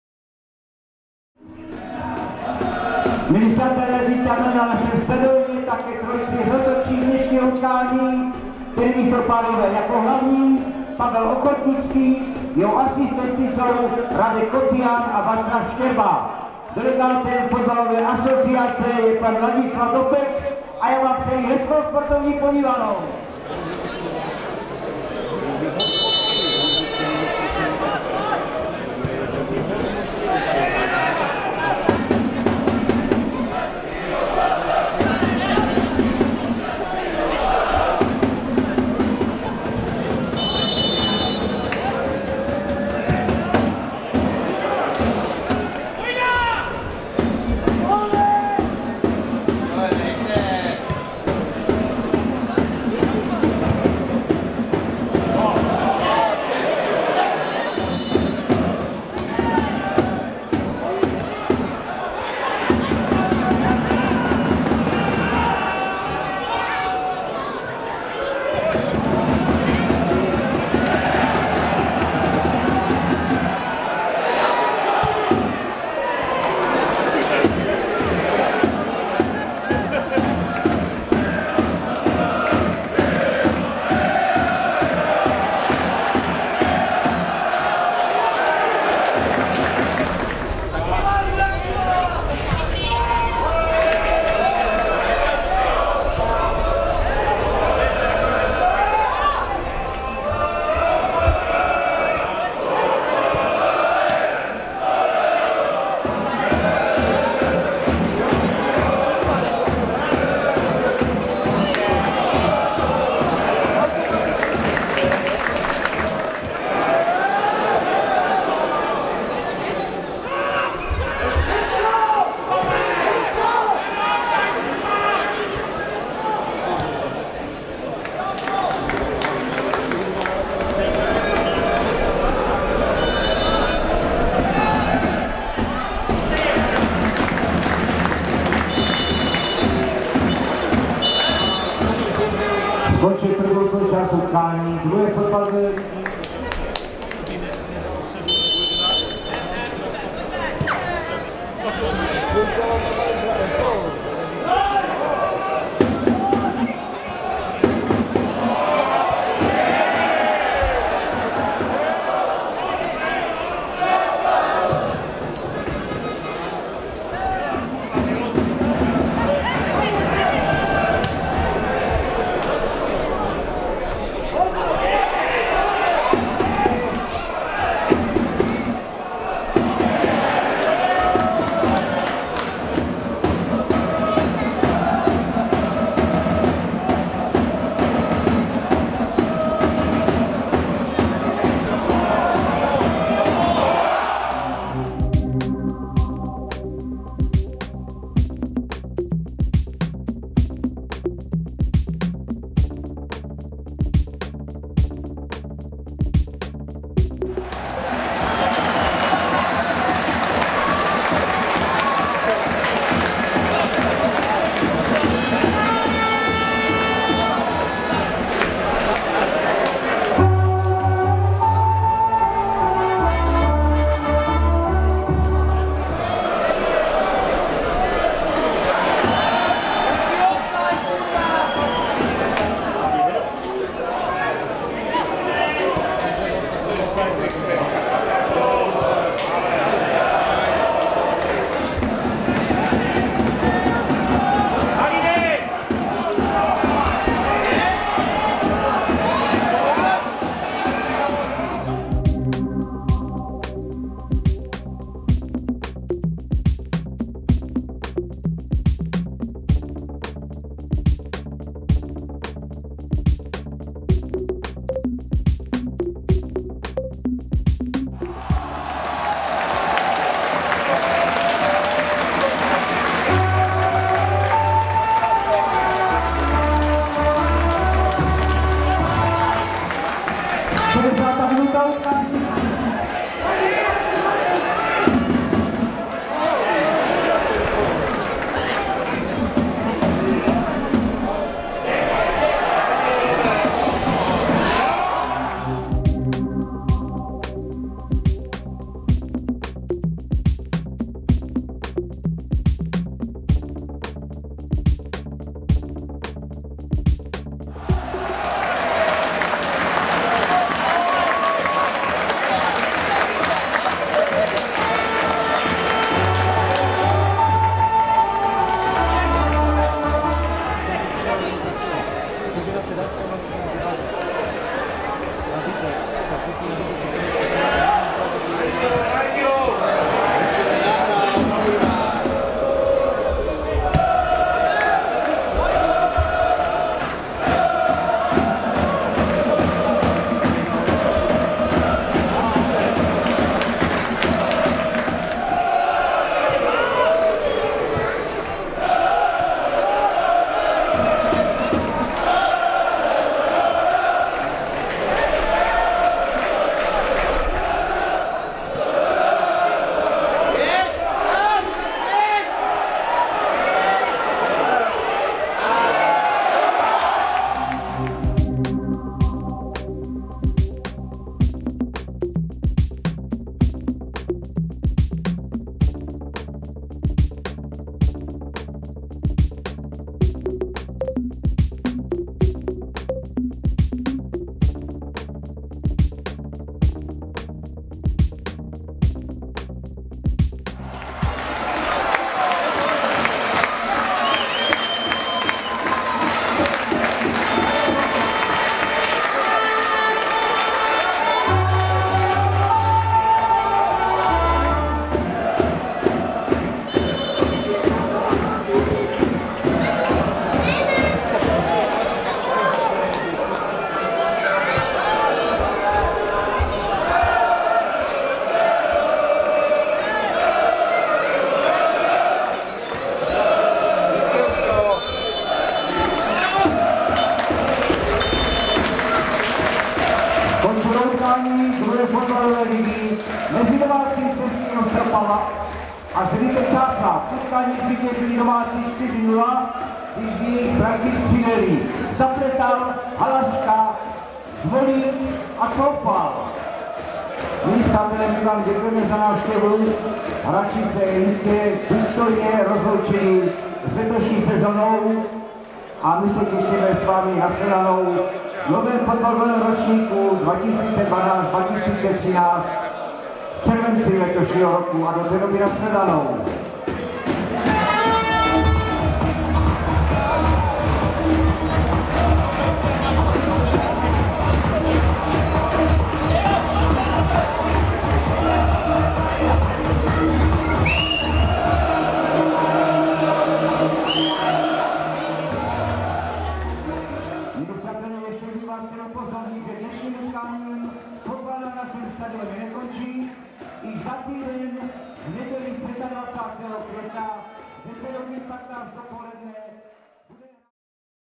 Slezský FC Opava - FC Zenit Čáslav
29. kolo II. fotbalové ligy 2011/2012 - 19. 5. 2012 - sestřih utkání